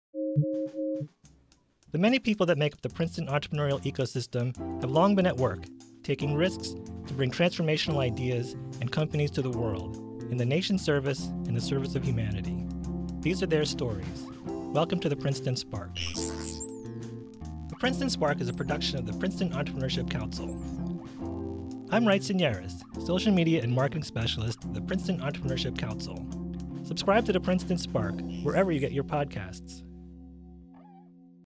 Princeton Spark Podcast Teaser
The various people that make up the Princeton entrepreneurial and innovation ecosystem have long been at work, taking risks to bring transformational ideas and companies to the world, in the Nation’s Service and the Service of Humanity. These are the stories of Entrepreneurship the Princeton Way.

Welcome to the Princeton Spark.

I’m your host, Wright Seneres, social media and marketing specialist at the Princeton Entrepreneurship Council.

Theme music by the Treadmills (Wright Seneres on guitar and bass, John Damond, Jr. on drums)